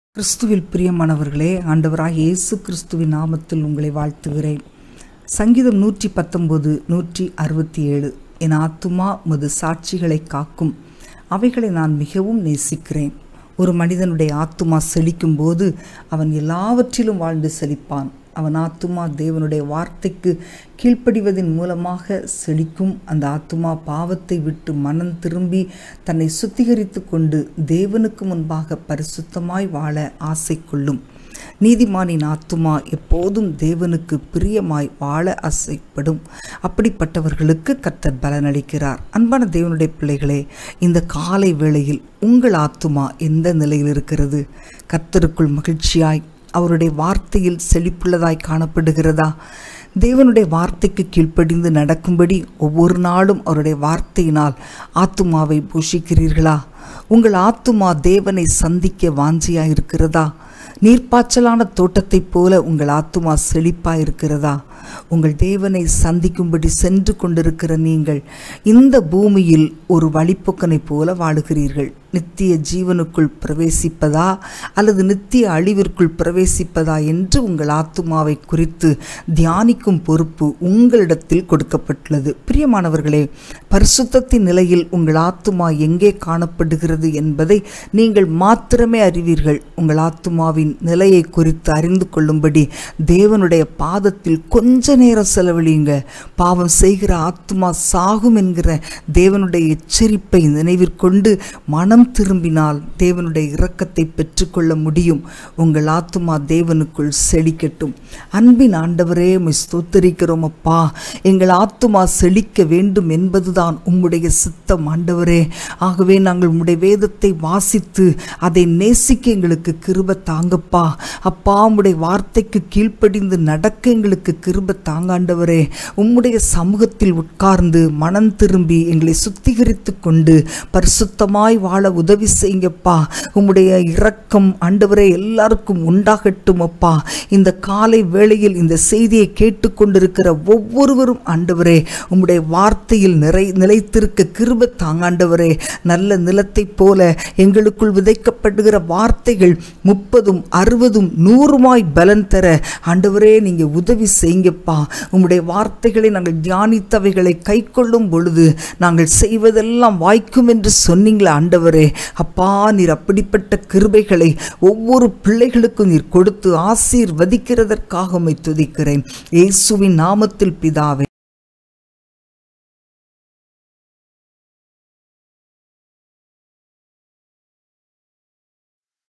Today's God's Word 10 06 2021
இன்றைய கர்த்தருடைய வார்த்தை. என் ஆத்துமா உமது சாட்சிகளைக் காக்கும். அவைகளை நான் மிகவும் நேசிக்கிறேன். சங்கீதம் 119:167. நீதிமானின் ஆத்துமா எப்போதும் தேவனுக்குப் பிரியமாய் வாழ ஆசைப்படும். அப்படிப் பட்டவர்களுக்குக் கர்த்தர் பலனளிக்கிறார். டார்லிங் சம்பத்
இந்தக் காலை வேளையில் உங்கள் ஆத்துமா எந்த நிலையிலிருக்கிறது? பரிசுத்தத்தின் நிலையில் உங்கள் ஆத்துமா எங்கே காணப்படுகிறது என்பதை நீங்கள் மாத்திரமே அறிவீர்கள். உங்கள் ஆத்துமாவின் நிலைமையைக் குறித்து அறிந்து கொள்ளும்படி தேவனுடைய பாதத்தில் சற்று நேரம் செலவழியுங்கள்.

பாவம் செய்கிற ஆத்துமா சாகும் என்கிற தேவனுடைய வார்த்தையின் எச்சரிப்பை நினைவிற் கொள்ளுங்கள். மனந்திரும்பினால் தேவனுடைய இரக்கத்தைப் பெற்றுக் கொள்வீர்கள். உங்கள் ஆத்துமா தேவனுக்குள் செழிக்கும்.

இன்றைய சிந்தனை. ஆத்துமா செழித்தோங்குவதே இந்த வேளையின் தேவை🙏🙏🙏

Today's God's Word. I obey Your statutes, for I love them greatly. Psalm 119:167. A soul that desires to please God is like good ground that receives God's Word. Darling Sambath

The soul of the righteous desires to please God. The Lord rewards them. How is the prosperity of your soul in this morning? Only you know the level of the purity that your soul carries today. Spend some time today pondering on the condition of your soul.

Remember the Word of God warns that the soul that sins dies but a repentant soul finds itself in the mercies of God. Your soul is flourishing in God.

Thought for the day. The prosperity of the soul is the need of the hour🙏🙏🙏